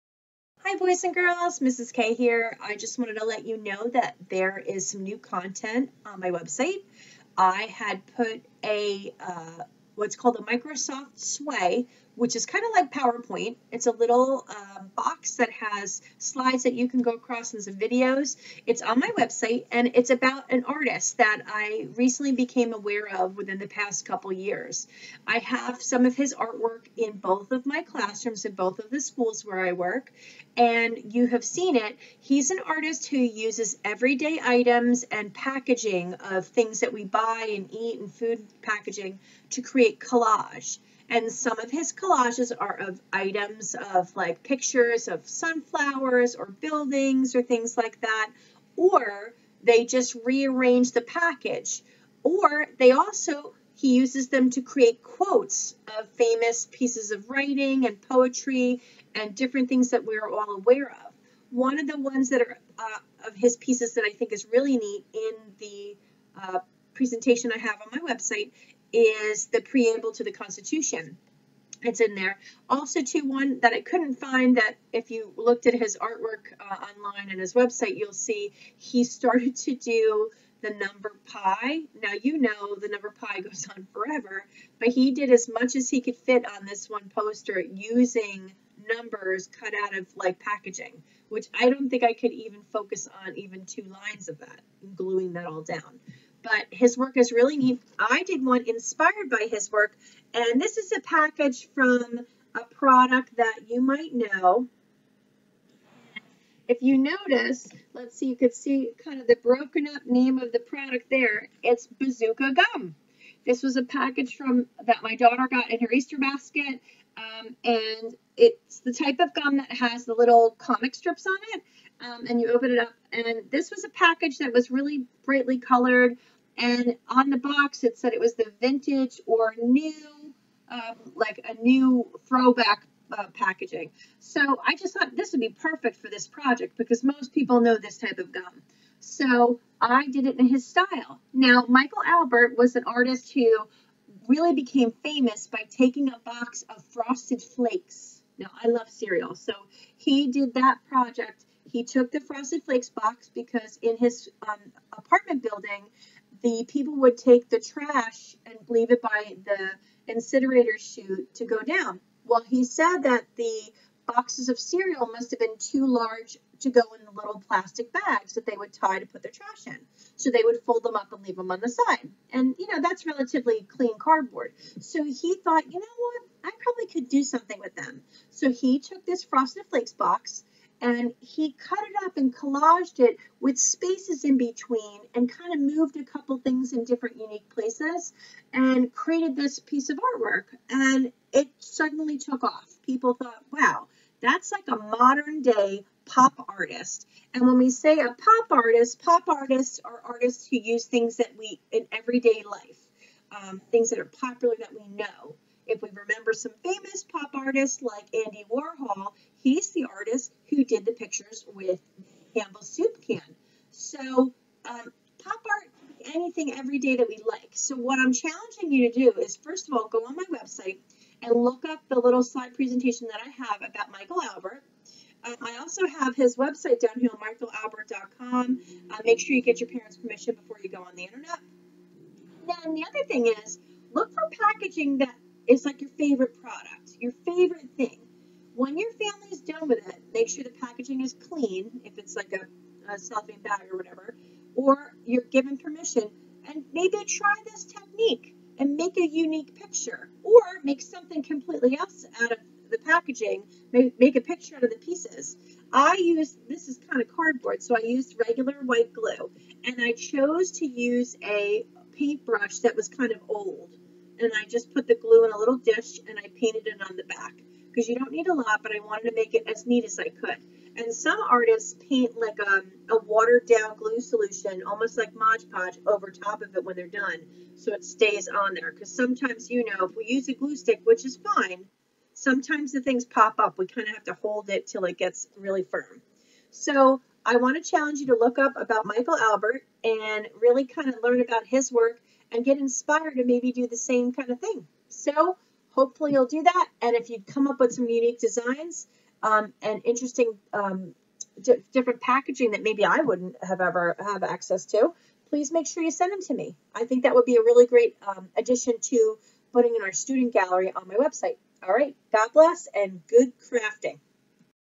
Pop Art Collage - Learn about modern American artist - Michael Albert
Why not try some Pop Art Collage??? Just take the packaging from something you have used up and cut it up into a new creation. Get those scissors and glue-sticks moving to create a new version of your favorite product. This is inspired by the art of a modern day Pop Artist named Michael Albert.
Get creating! Please share photos of your work with me through my school email.